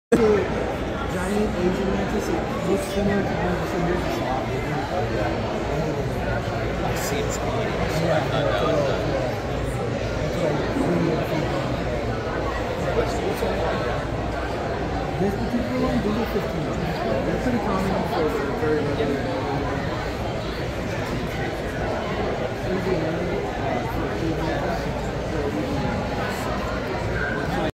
Repticon Nov. 2022 (Orange County) Costa Mesa, California
Part 2.
Repticon Costa Mesa

Also had my brother put his art/stickers under my mantis tree.